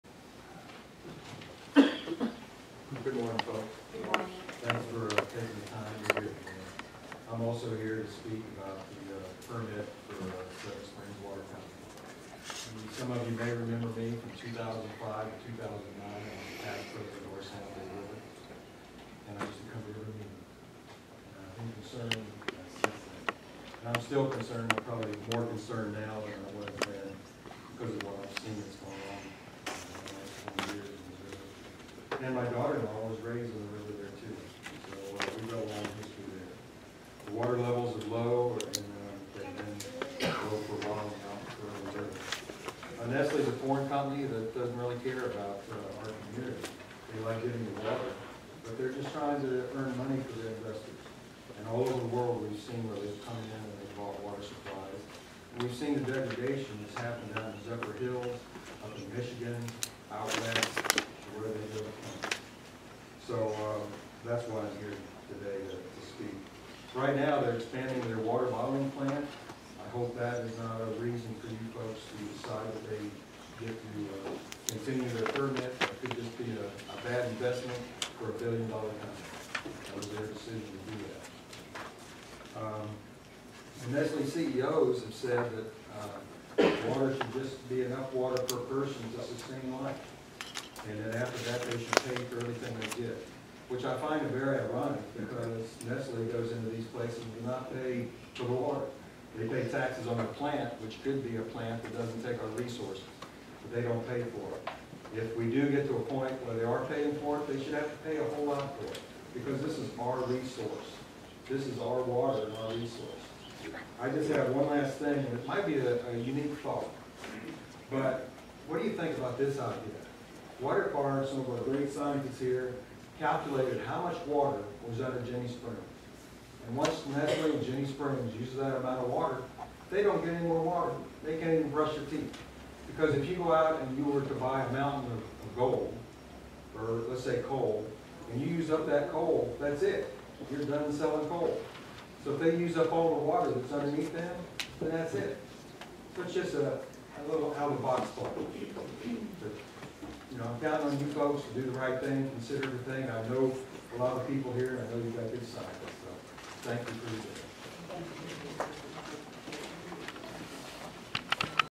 Nestle just wants money for investors extracted from our waters --Russ Augsburg @ SRWMD 2019-12-10
Citizens against Nestle at Suwannee River Water Management District Board, December 10, 2019,